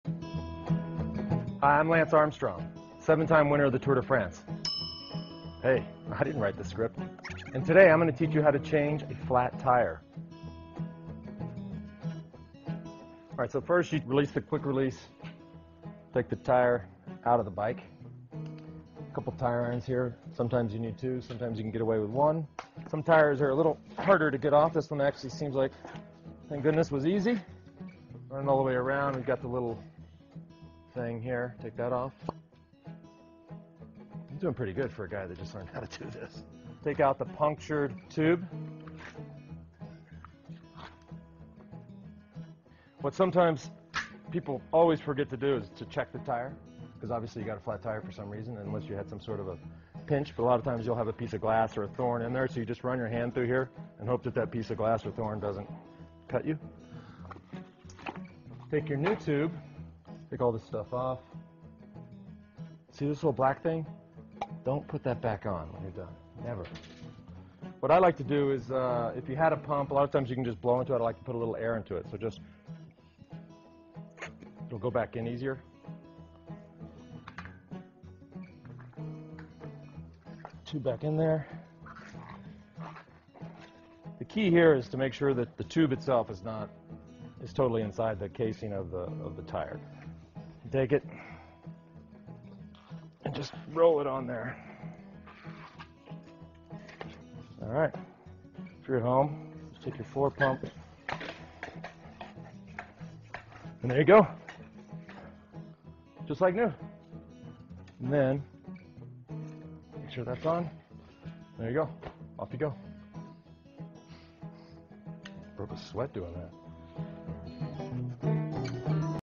Lance Armstrong Appears In Funny Instructional Video
In what may be one of Lance Armstrong's more quirky attempts to salvage his image, the cyclist has appeared in an informational video for Outside Magazine showing it's online readers how to fix a flat tire on a bicycle.

"Hi. I'm Lance Armstrong. Seven times winner of the Tour de France" the cyclist announces at the top of the video before offering the disclaimer "Hey, I didn't write the script."

The video was included in an issue of the magazine that featured numerous how-to articles.

In an earlier attempt at image fixing, some 28 million viewers worldwide watched Oprah Winfrey's two-part interview in which Armstrong finally admitted he had been taking performance-enhancing substances for all seven of his Tour de France victories.

"Back in the work world. The clothes are more comfortable and I get two breaks a shift. Video from first day on the job" is how Lance Armstrong describes the appearance on his Facebook page.

The cyclist was handed a lifetime ban and stripped of his seven Tour de France titles in 2012 after an extended U.S. Anti-Doping Agency investigation.

U.S. Federal prosecutors are seeking to recover $40 million paid by the U.S. Postal Service, alleging taxpayer money was used to finance Armstrong's doping -- explicitly forbidden in his government sponsorship contract. The suit is a whistleblower suit first brought by Armstrong's ex-teammate Floyd Landis.

Outside Magazine, in promoting the Armstrong video this week took to twitter to announce the appearance

"What's @lancearmstrong to do after the worst crash in sports history? Start at the bottom and work his way up again."